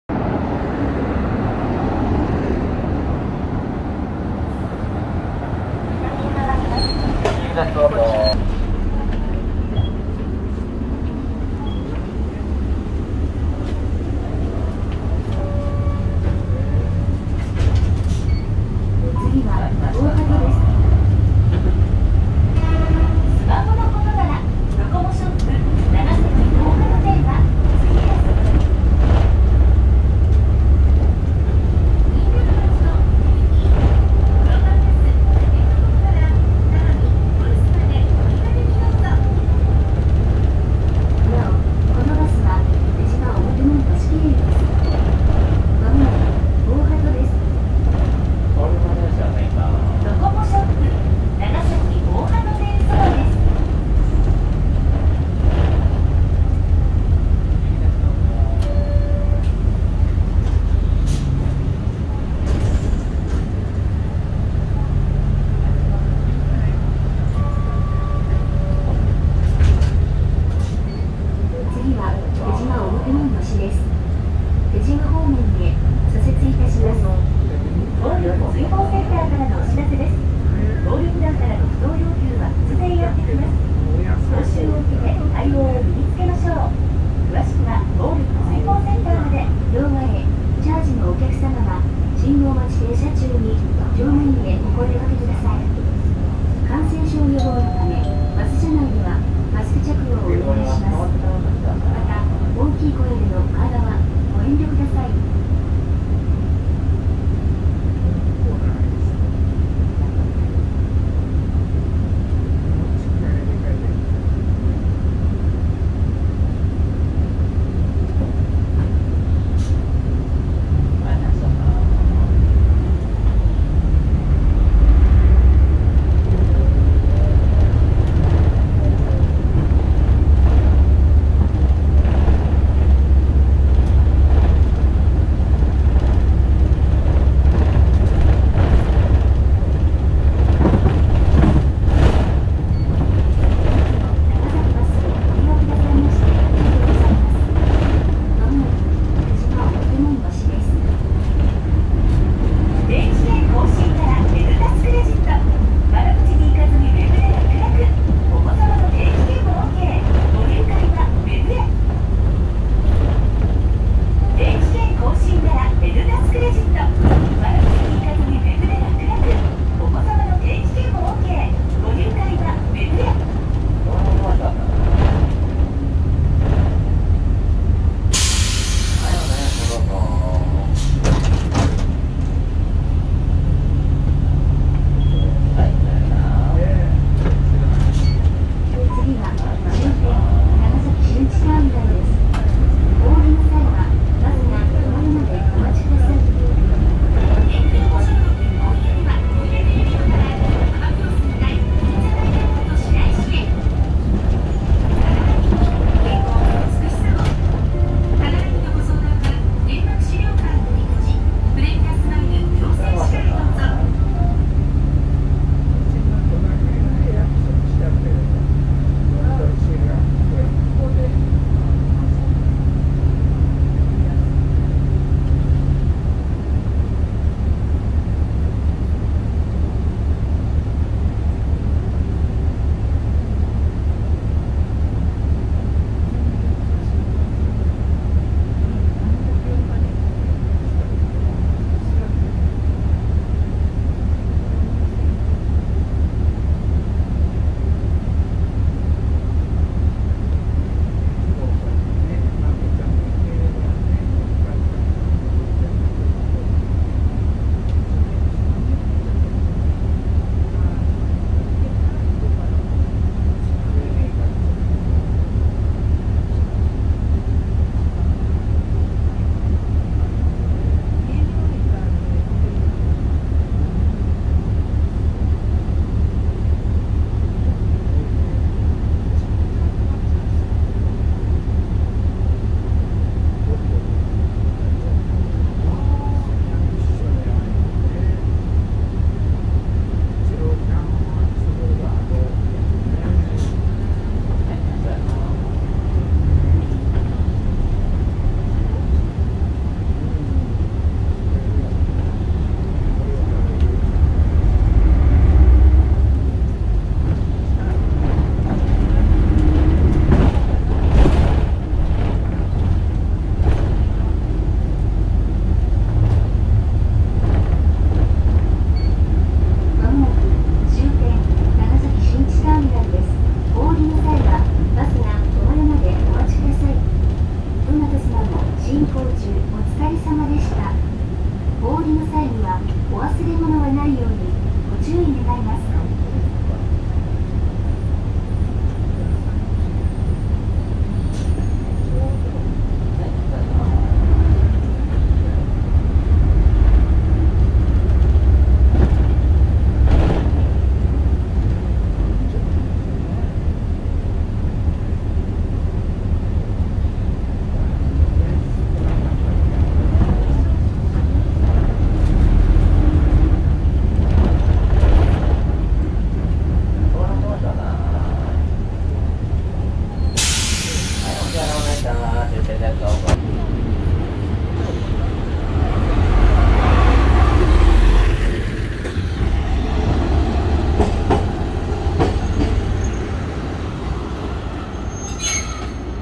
【いすゞキュービック】長崎バス 前面展望・車窓 20番系統 五島町→長崎新地ターミナル 1625号車 KC-LV380N【1996年式 (平成8年式)】
初めてキュービックに乗りました。昔は地元の京浜急行バスにもいたそうですが、全然記憶がないです。久里浜営業所にも10年ちょっと前までいたそうです。

この車両は桜の里営業所の所属みたいです。